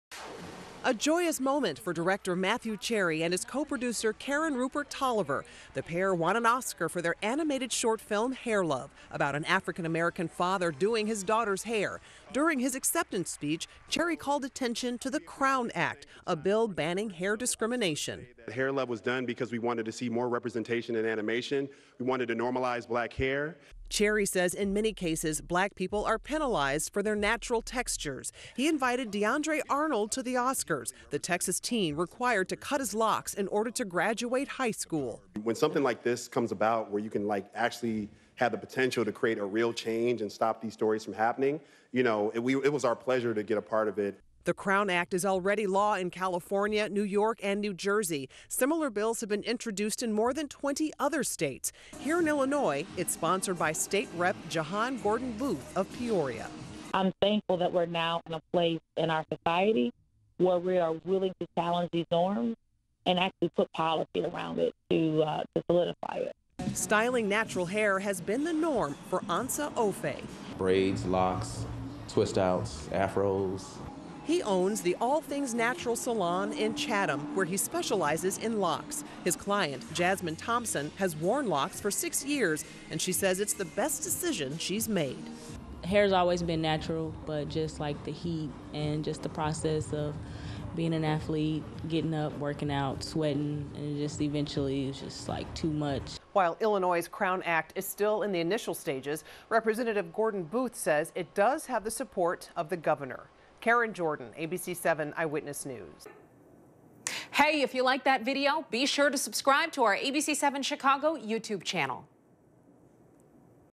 Crown Act proponents hope 'Hair Love' win will give anti-discrimination legislation momentum
There's hope that the Oscar win for "Hair Love" will give momentum to legislation called the Crown Act that protects against discrimination in the workplace, housing and education based on hair texture and protective styles.